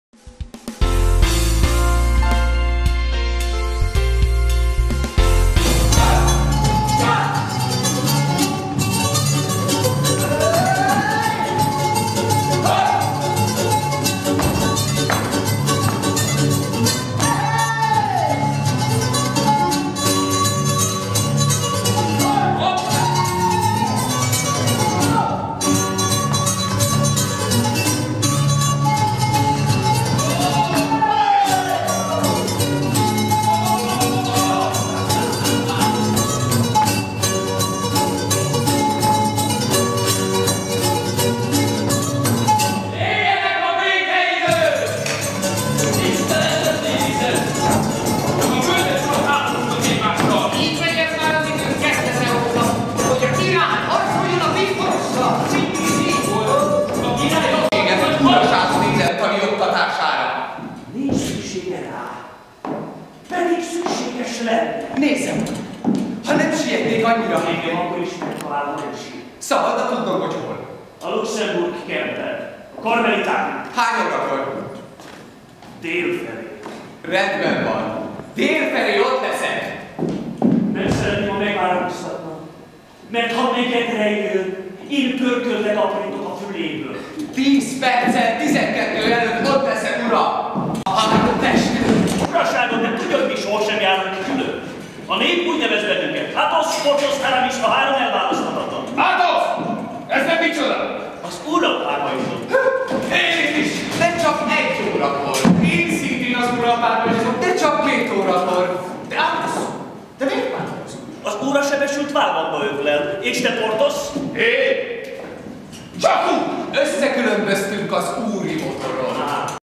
A három testőr a Pannon Várszínházban
Alexandre Dumas A három testőr című regényének színpadi változatát mutatja be a Pannon Várszínház a Hangvillában.